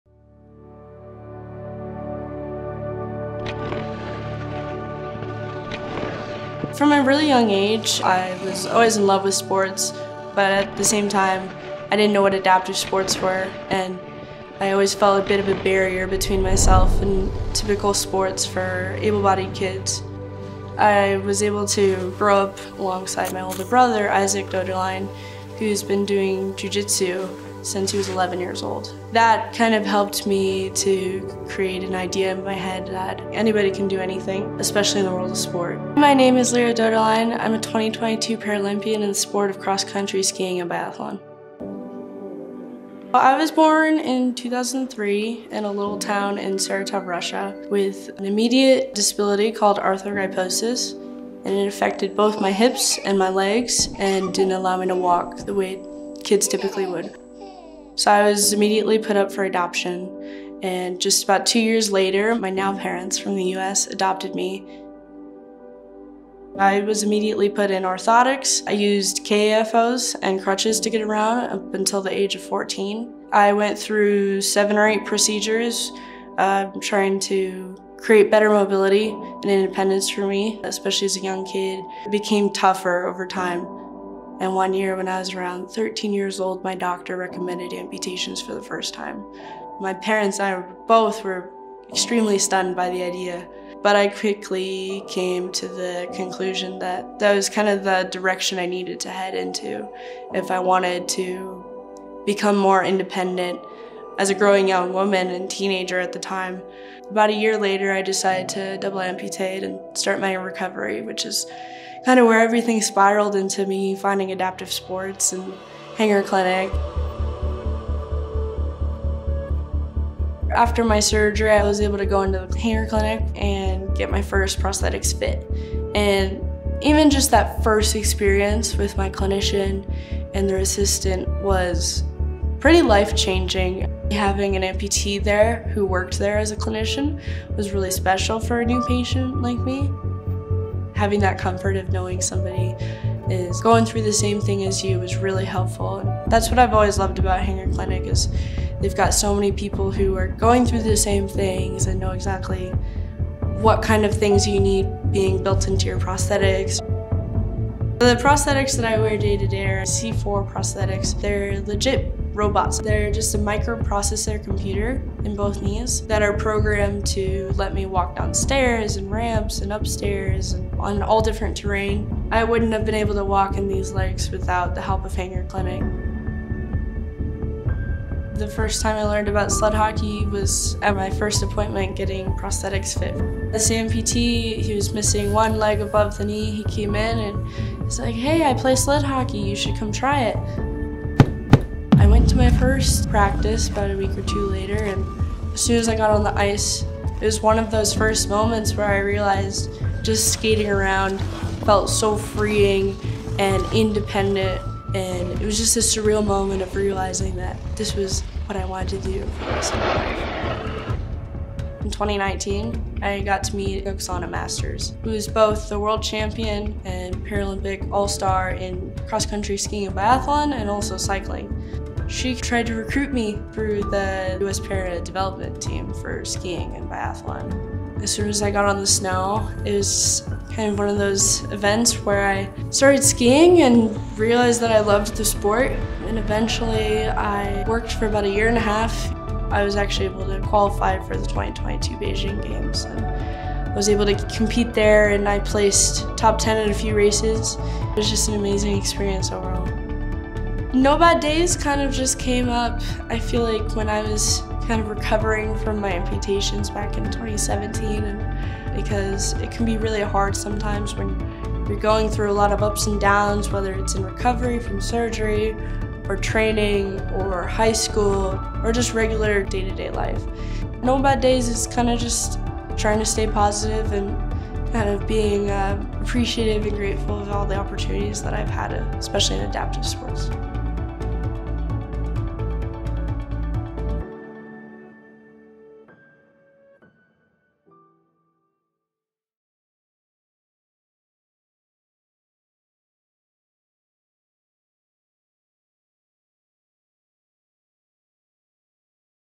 Lera Doederlein Overcomes Arthrogryposis to Become Paralympian and USA Hockey Development Player
With bilateral above-knee prostheses, Lera is achieving her mobility goals while pursuing adaptive sports and participating on the women's development team for USA Hockey.

Lera was born in Saratov, Russia with a congenital condition called arthrogryposis of the lower extremities. While a teenager, Lera and her family would make the difficult decision to amputate so she could achieve better mobility with the use of bilateral above-knee prostheses. After learning to walk again, Lera quickly developed a passion for adaptive sports, including cross country skiing and sled hockey. Since that time, Lera has become an inspiration to other bilateral amputees. She recently participated in Hanger Live's inaugural sled hockey event and is currently on the women's development team for USA Hockey.